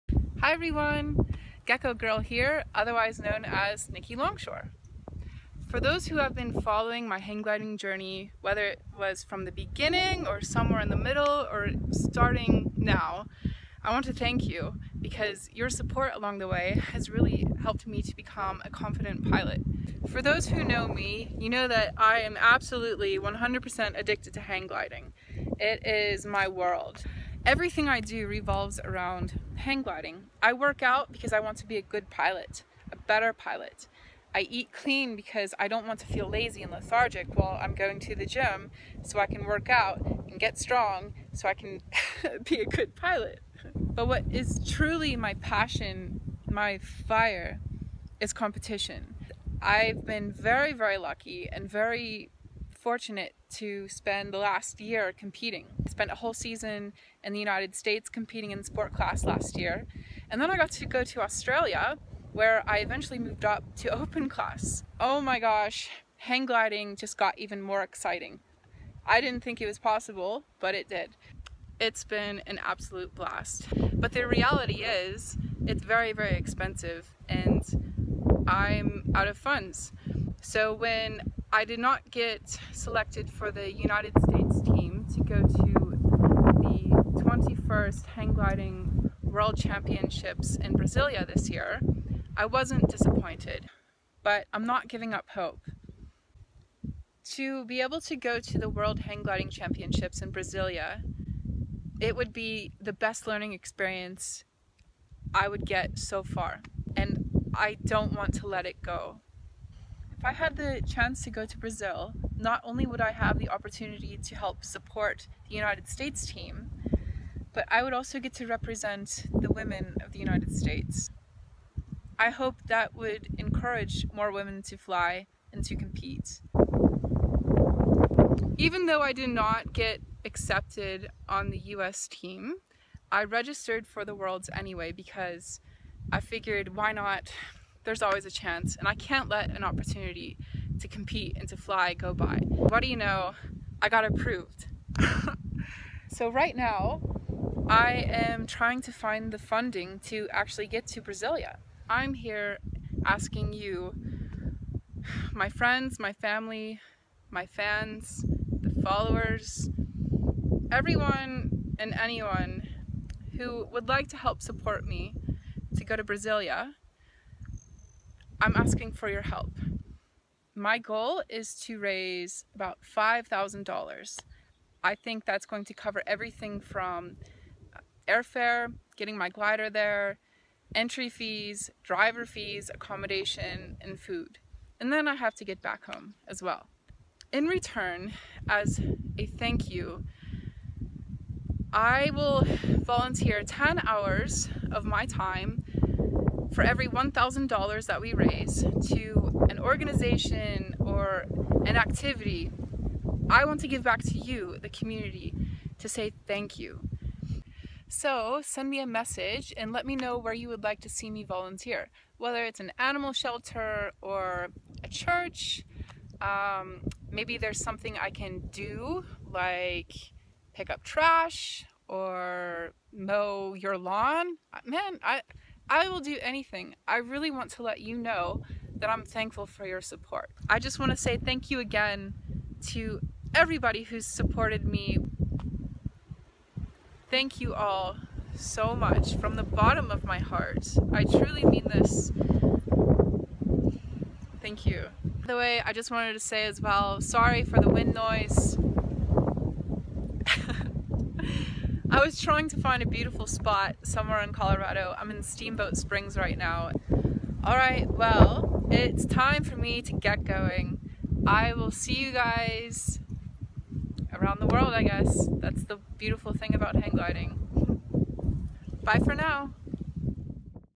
Send Gecko Girl to the Worlds!
Hi Everyone! Gecko Girl here ;) I am trying to make it to the 21st Hang Gliding World Championships in Brasilia this year, but I need your help!...